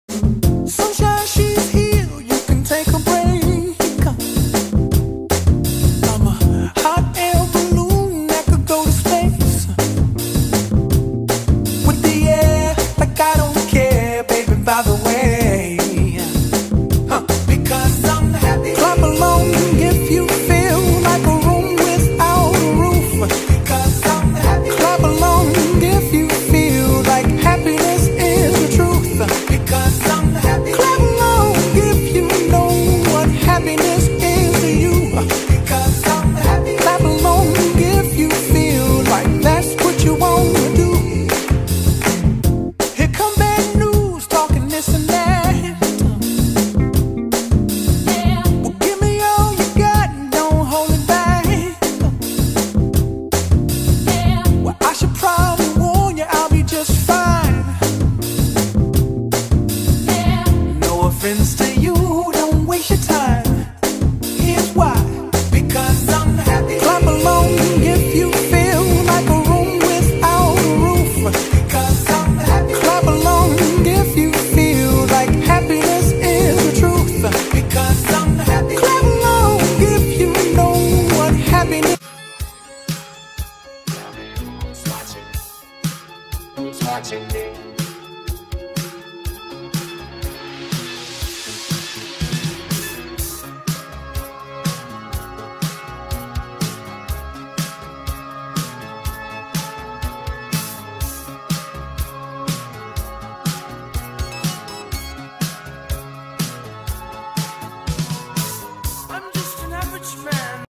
Swag Dog eating with human hands
Rhodesian ridgeback eating with human hands